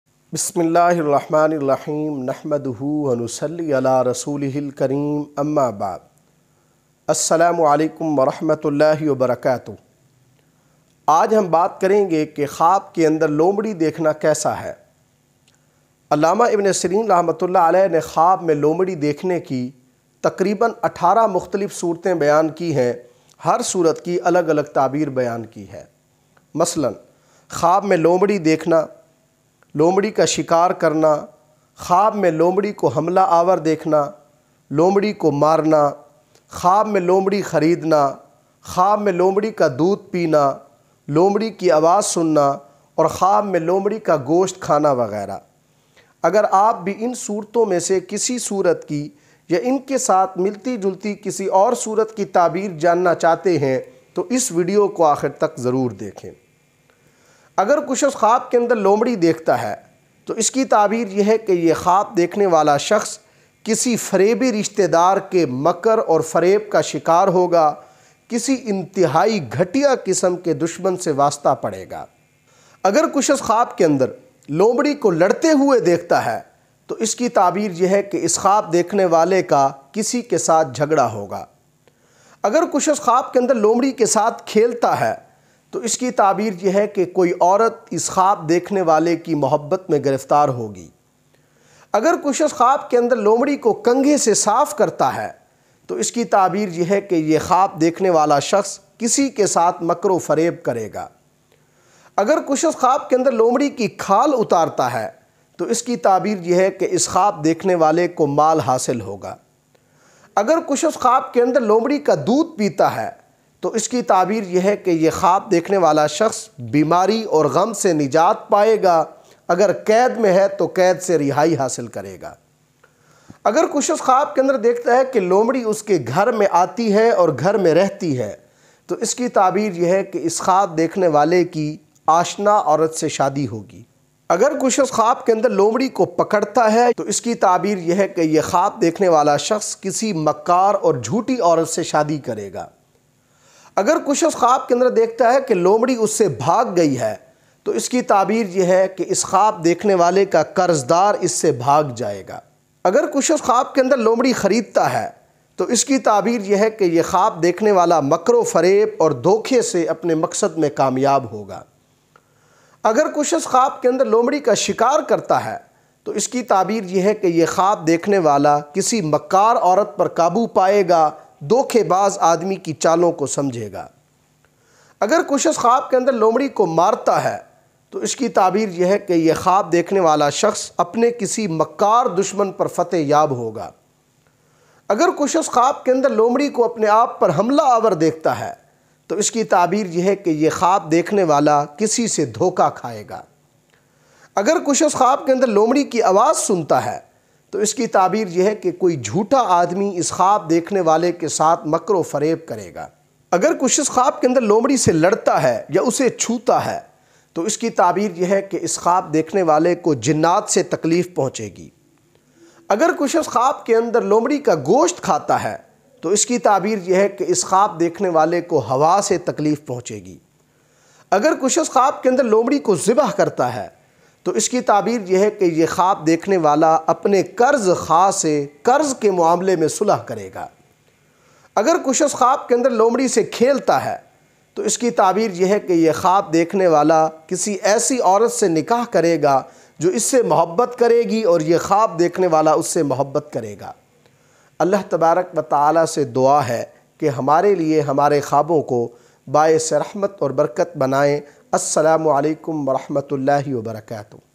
khwab mein lomri dekhna | Fox dream meaning | خواب میں لومڑی دیکھنے کی تعبیر
khwab mein lomri dekhna | Fox dream meaning | خواب میں لومڑی دیکھنے کی تعبیر is vidio main khwab mien lomri dekhna
Khwab mein fox dekhna
Khwab mein loomri dekhna ki tabeer byan ki gai hai
Khwab mein lomri dekhna ki 18 mukhtalif sorten hain har sorat ki alag alag tabeer byan ki gai hai

This video explains the meaning of seeing a fox in a dream